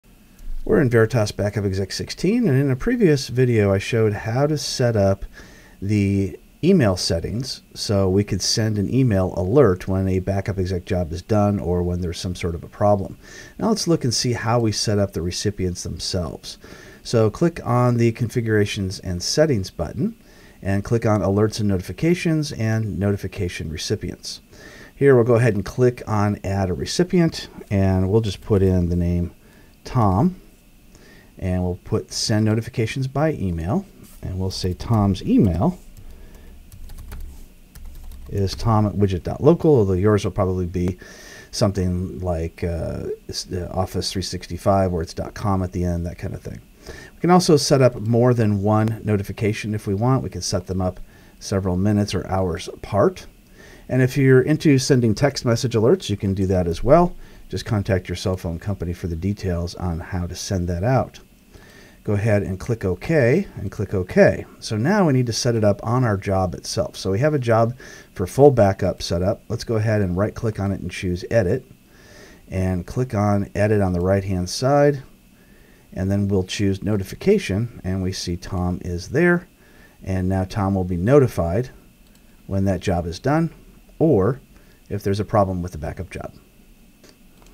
How to setup an email recipient in Veritas Backup Exec 16
Professor Robert McMillen shows you how to setup an email recipient in Veritas Backup Exec 16.